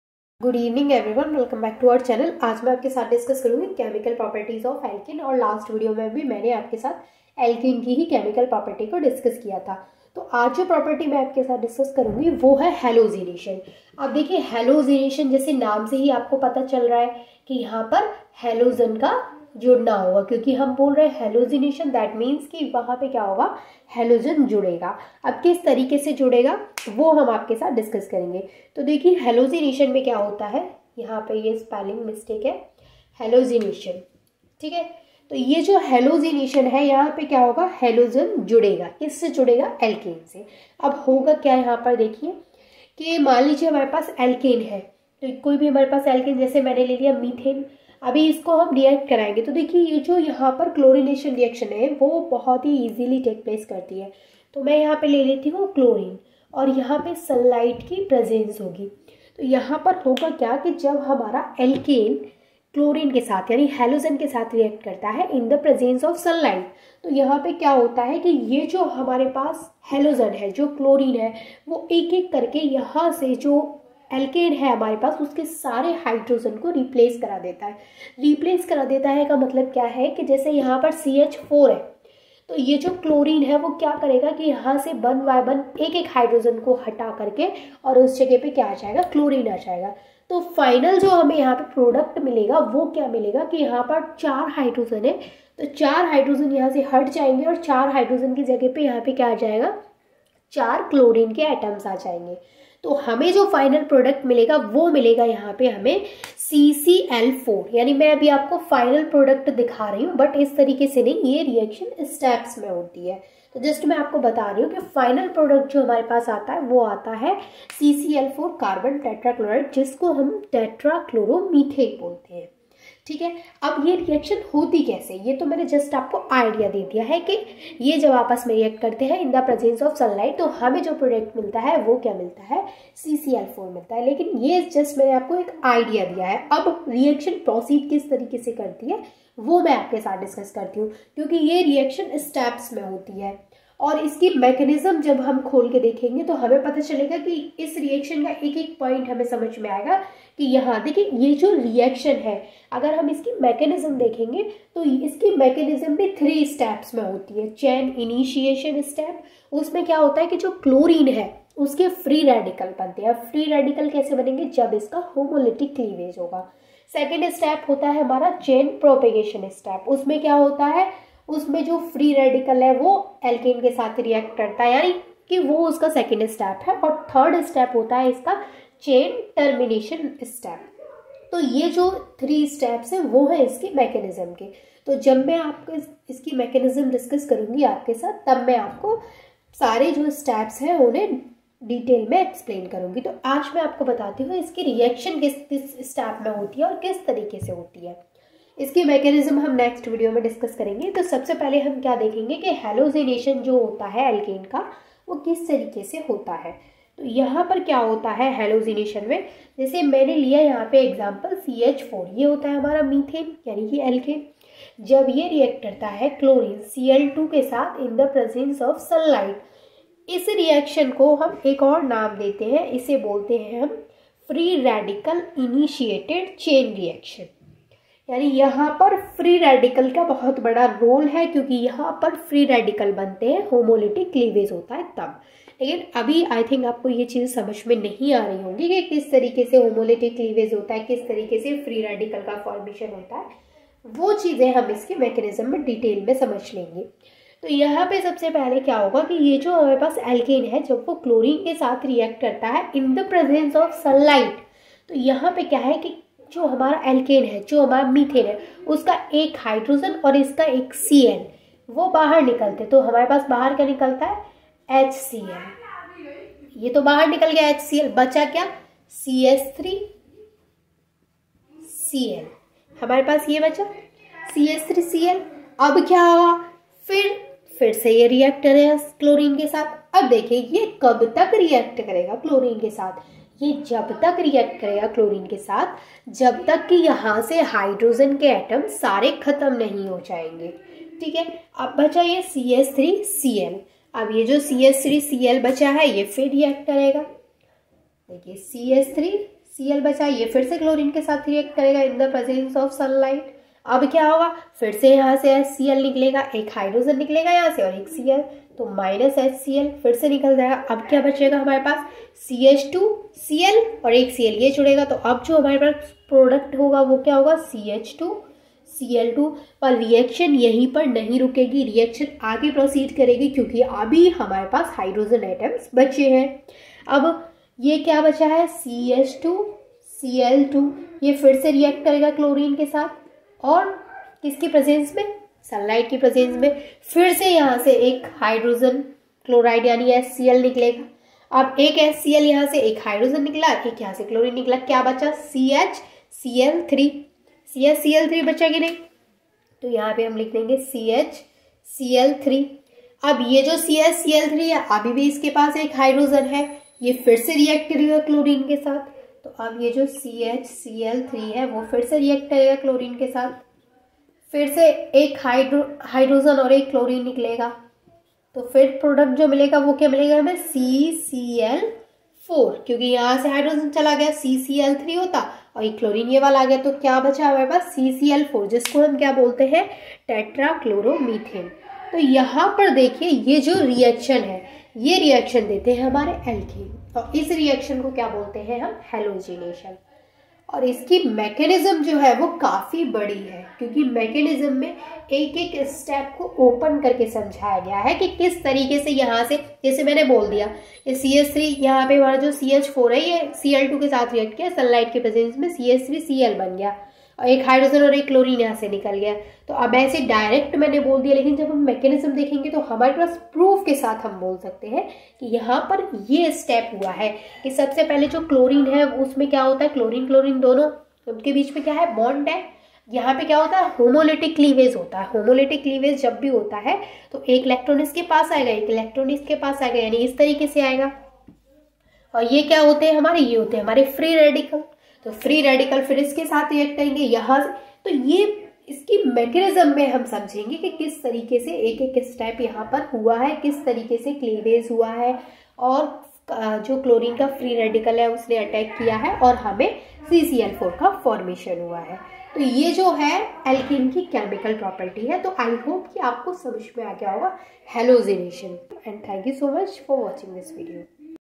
Chemical properties of Alkane | Halogenation reaction of Alkane | bsc 5th semester
• Today we will learn about "Chemical properties of Alkane"In this lacture. Find all the lacture of Bsc 2nd year, 3rd semester...

If you have any doubt you can ask in below comment box.

[ Bsc 1st year ] •• •• ••

Social links -
coming soon